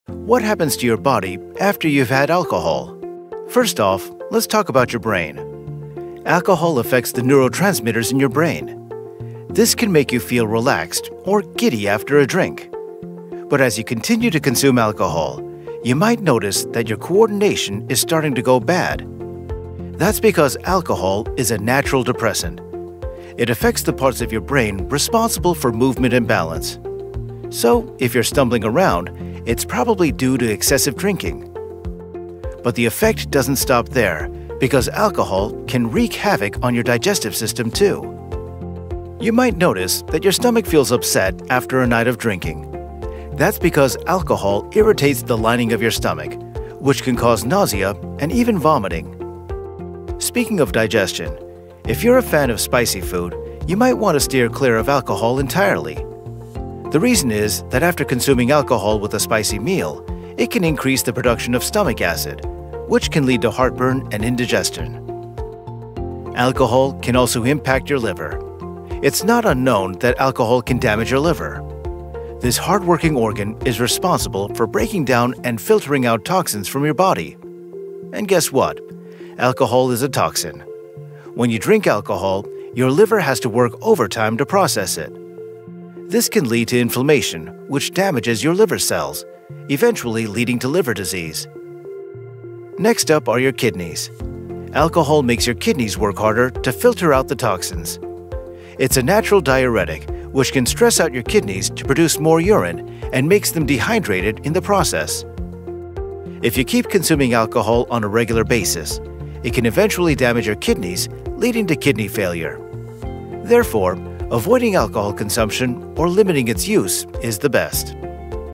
What happens when you drink Alcohol
Ever wondered what happens to your body when you drink alcohol? In this video, we dive deep into the effects of alcohol on your brain, liver, and overall health. Learn about the short-term and long-term impacts, and discover why moderation is key. Whether you’re curious about the science behind alcohol or looking for ways to make healthier choices, this video has you covered.

Other Video Links: 👇👇
Early Signs & Symptoms of A Heart Attack